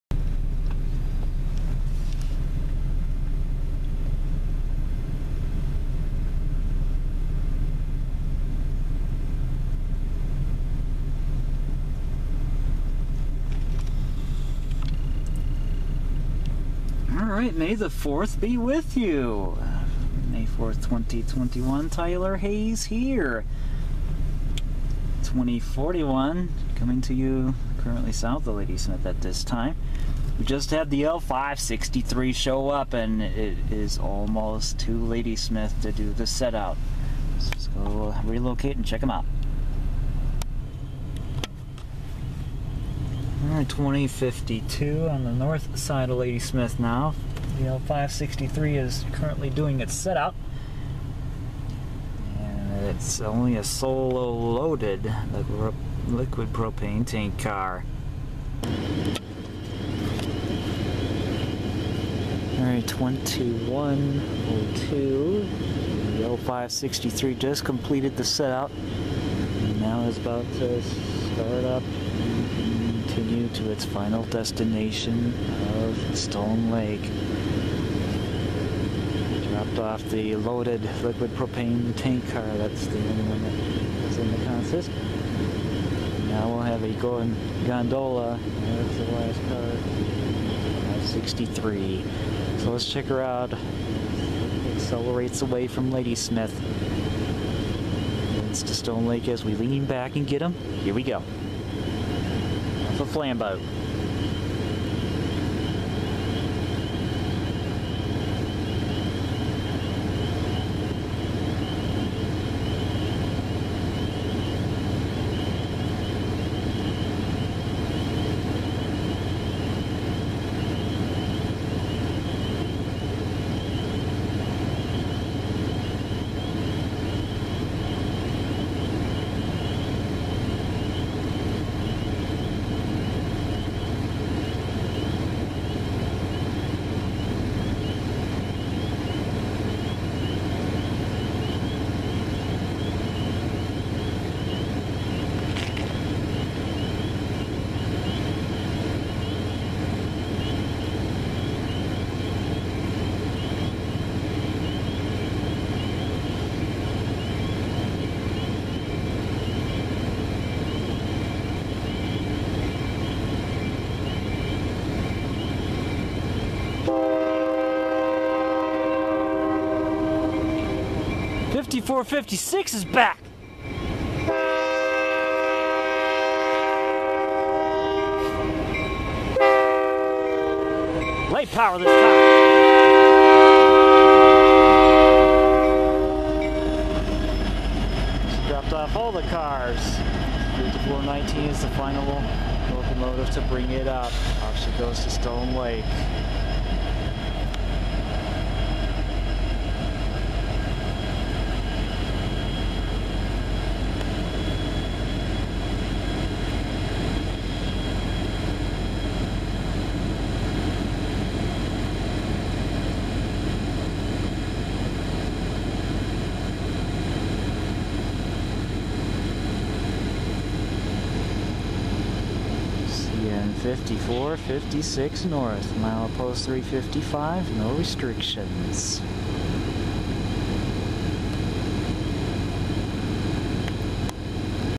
CN 5456 NB "Woodtick" L563 around Ladysmith on 5-4-21
Welcome back yet again to the channel friends! So glad you're still tuning in as I have the next video lined up for you. Had the L563 show up on the south side of Ladysmith as it was less than a couple miles to Ladysmith Yard to set out the remaining consist it had in tow on the arrival. This was at dusk. Little bit later I positioned myself on the north side off of Flambeau Avenue for the departure lite as 563 dropped the cars off at Ladysmith Yard and see the lite power depart and head for its destination of Stone Lake. The SD60 duo of CN 5456 and CN 5419 did the honors that evening.